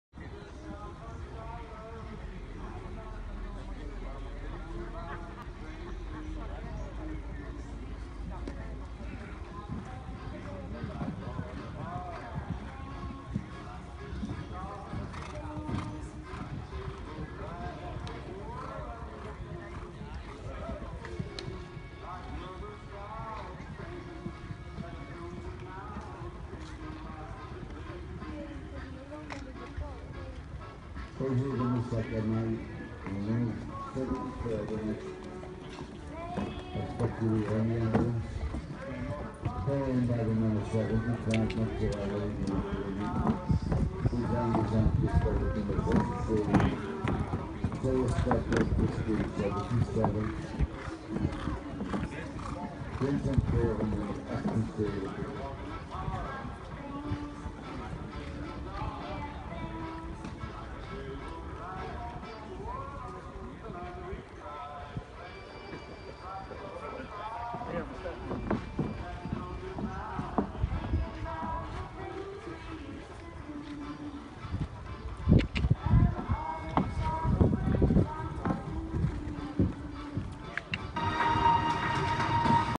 Amarit d'Amour CSI3* 1.50m Longines Ranking class Sentower Park 14.8.2017
Amarit d'Amour (gelding by Ashby x Damiani born 2009) - in 3* 1.50m Longines Ranking class Sentower Park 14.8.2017 finishing 8th